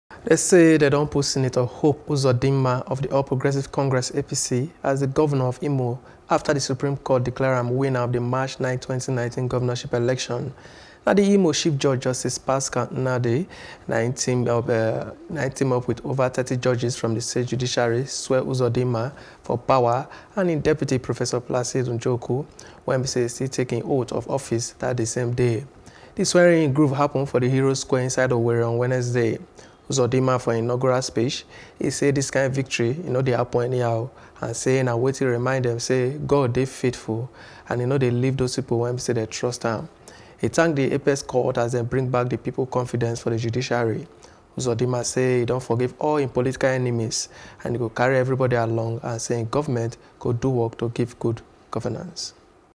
Dem Don Swear In Sen. Hope Uzodinma As Governor
Dem say dem don put Sen. Hope Uzodinma of the All Progressives Congress (APC) as the governor of Imo after the Supreme Court declare am winner of the March 9, 2019 governorship election.